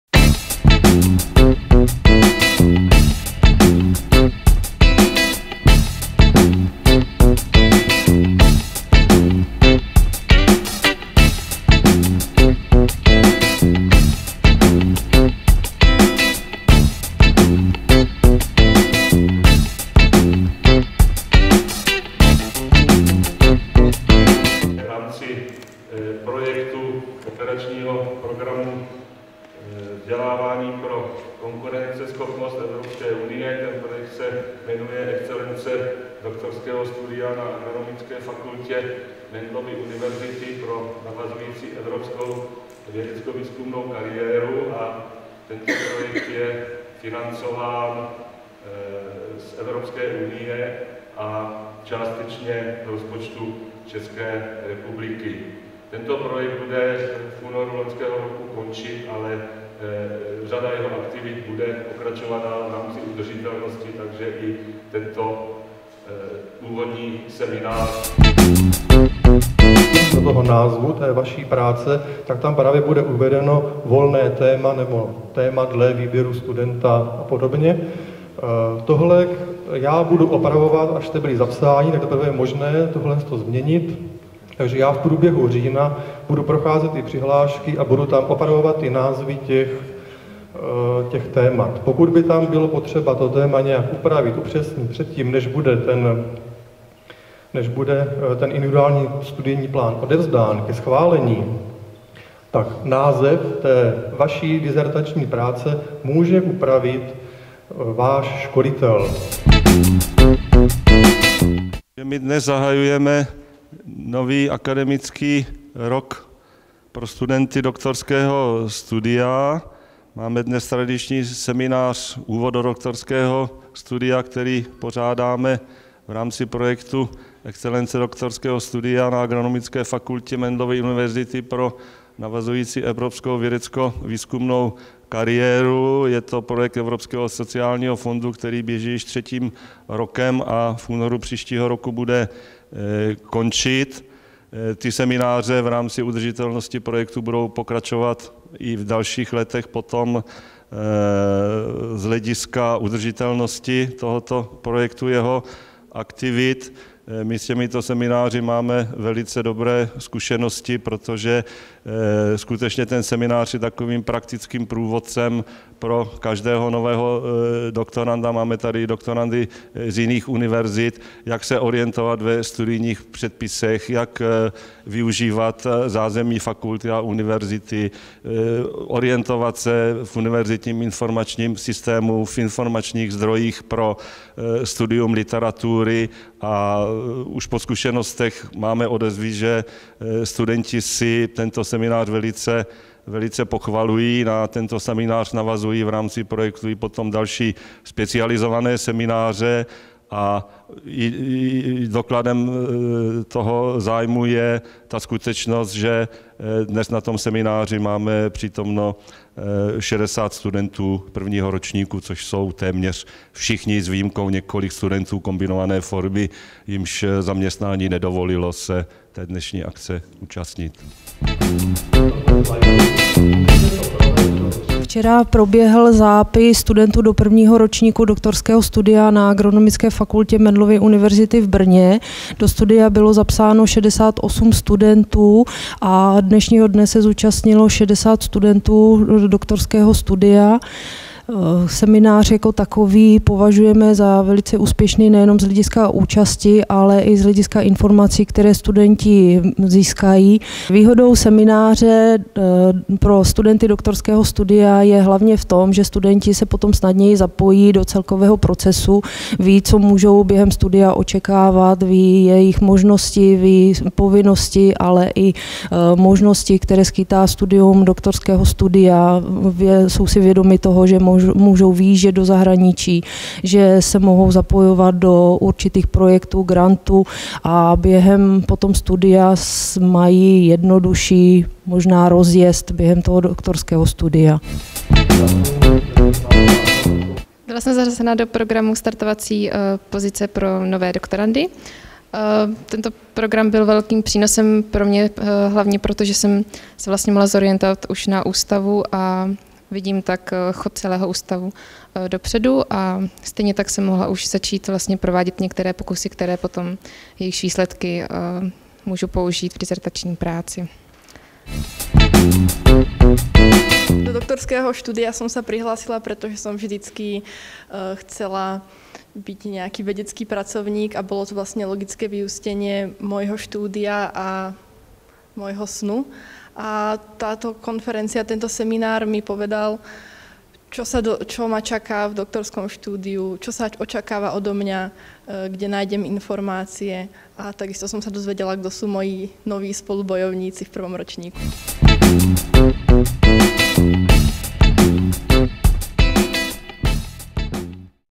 Úvod do doktorského studia AF MENDELU
Mendelova univerzita v Brně, aula, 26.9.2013
Excelence doktorského studia na AF MENDELU
pro navazující evropskou vědecko - výzkumnou kariéru
CZ.1.07/2.3.00/20.0005